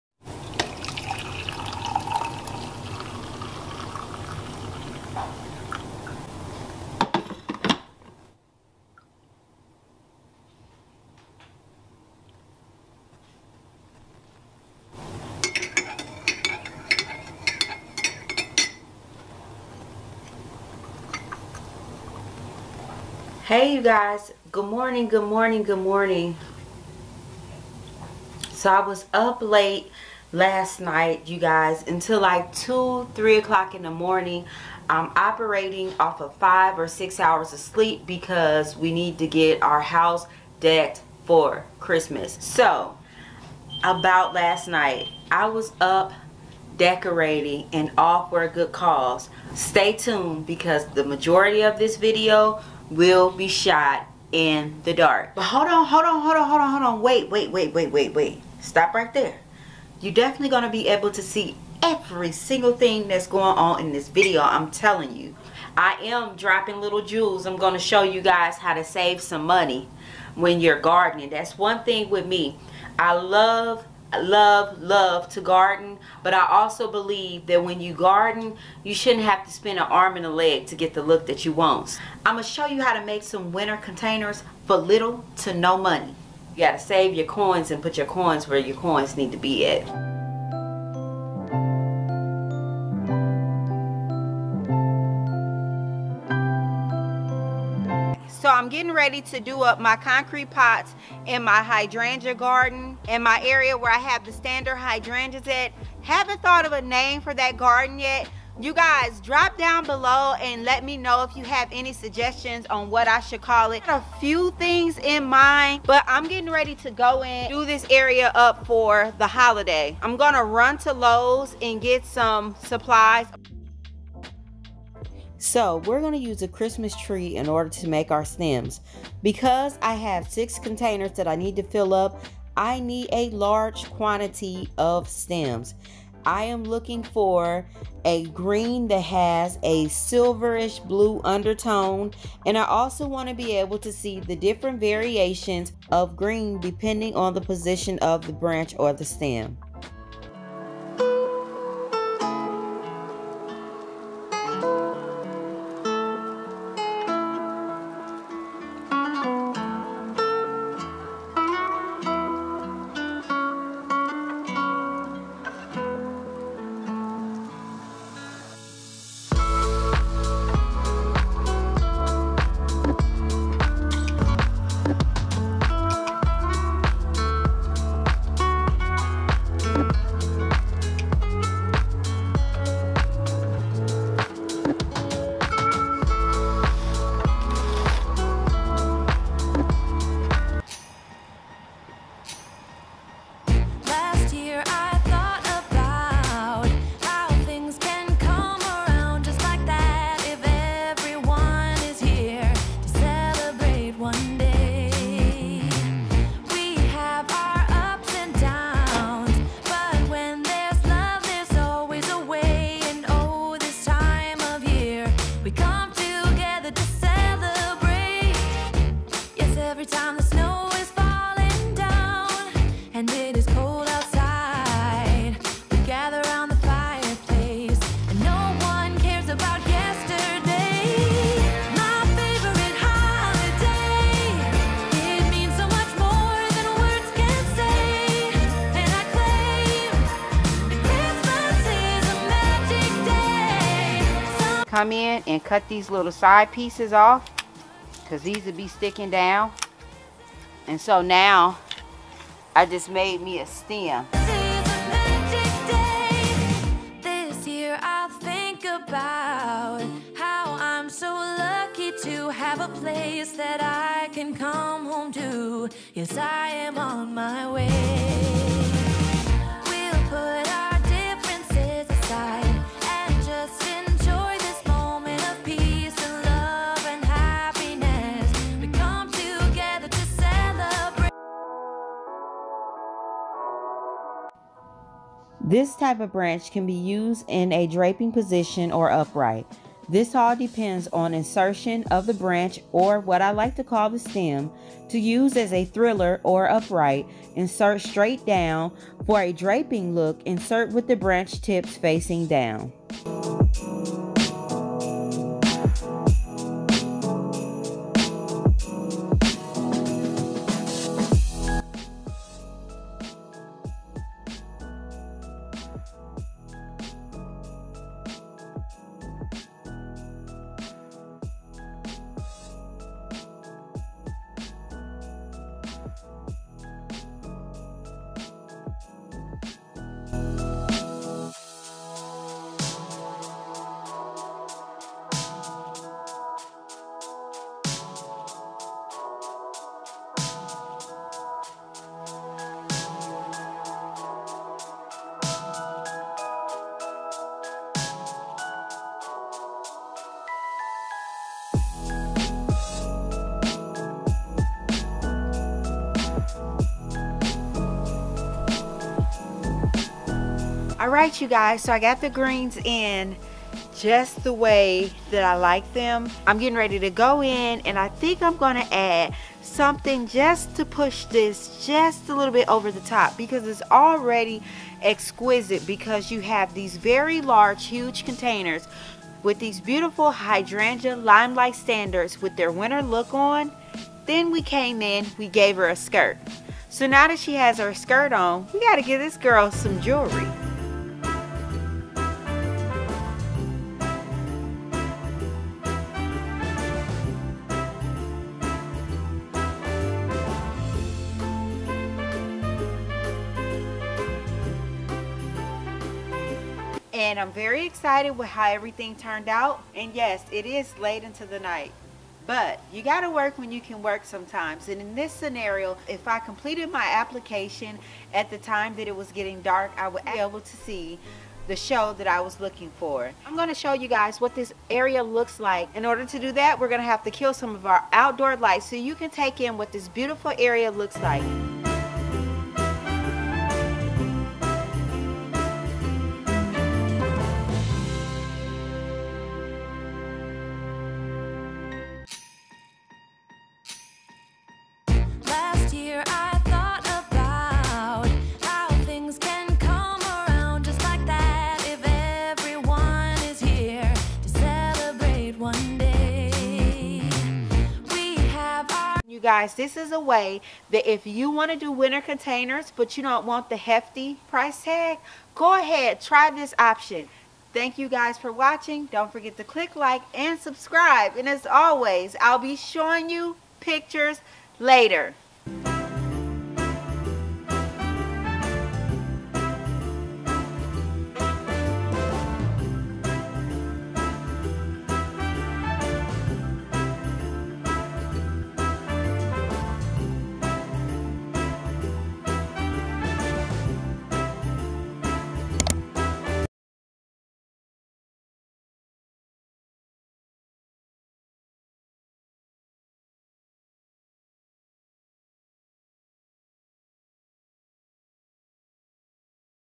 WINTER CONTAINERS // HOW TO SAVE MONEY ON WINTER CONTAINERS
WINTER CONTAINERS // HOW TO SAVE MONEY ON WINTER CONTAINERS // GARDENVLOG

Hey you guys winter containers can be so expensive especially when you plant up more than a pair! I will show you guys a little trick I like to use when planting up a large quantity of containers!!.